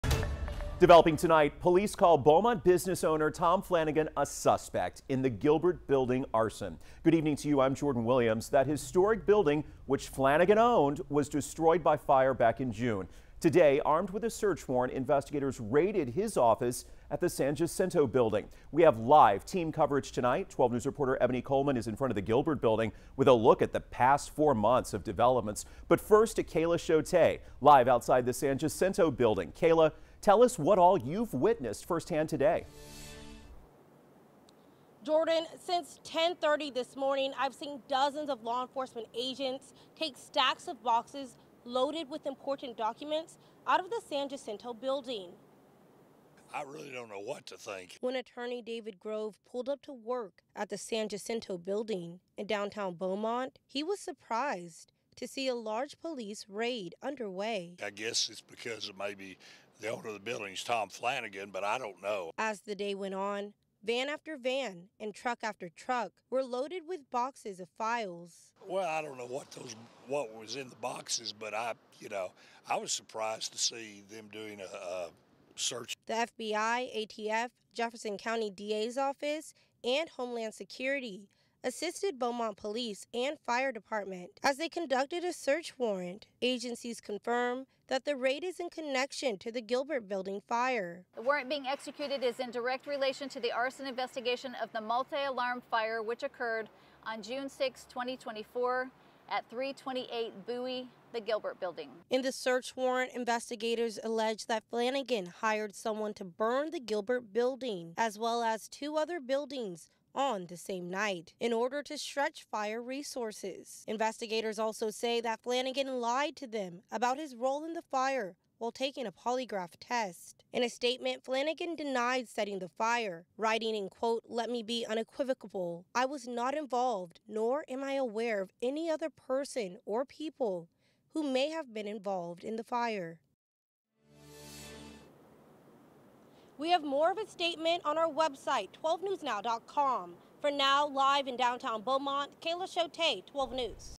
Beaumont developer Tom Flanagan named as arson suspect in Gilbert Building fire
Flanagan owns the Gilbert Building which was gutted by a fire in June 2024.